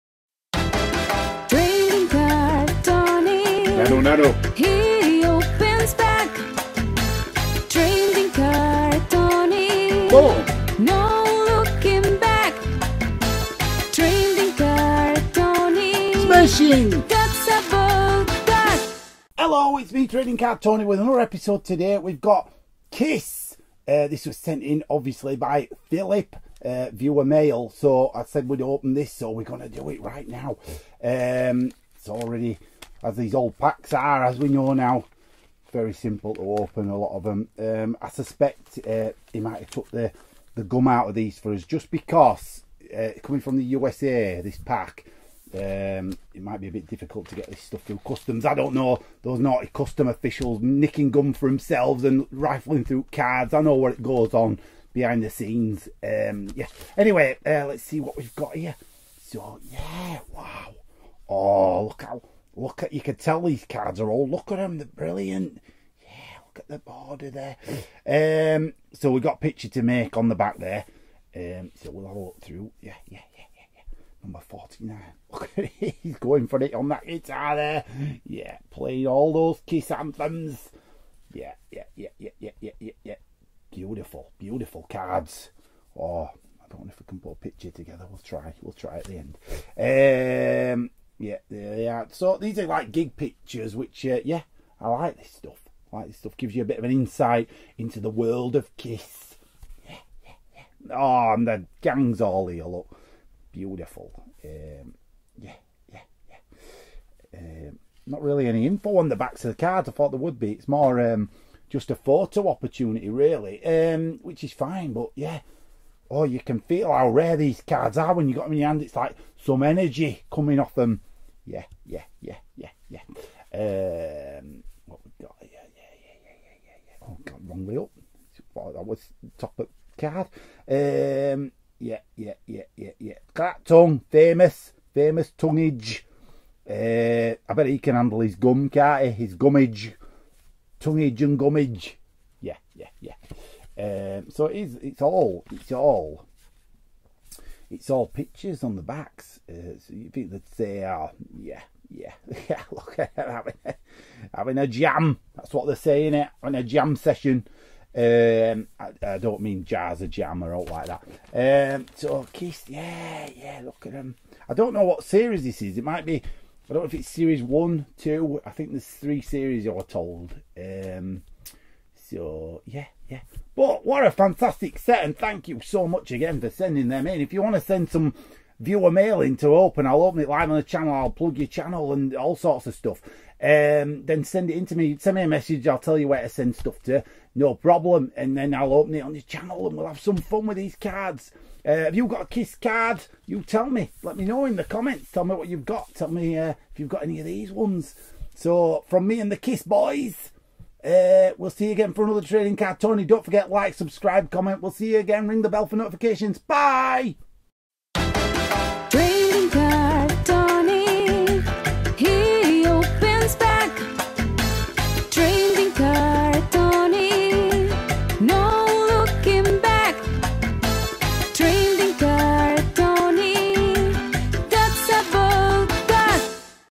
Trading Card Tony #108 - Kiss - Pack Opening!
The show where Trading Card Tony opens trading card packs and offers trading card tips on how to handle your trading cards!